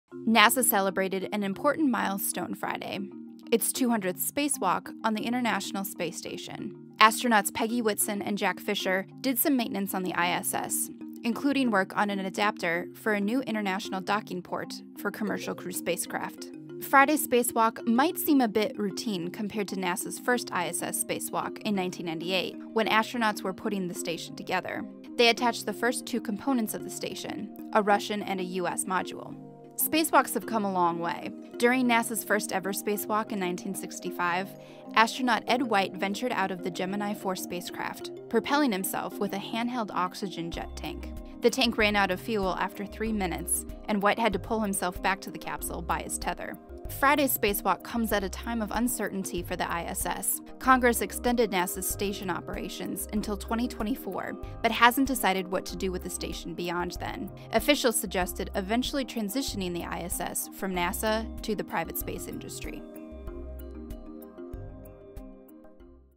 NASA completes milestone 200th ISS spacewalk
Two NASA astronauts did some maintenance on the International Space Station, including preparations for future commercial crew spacecraft.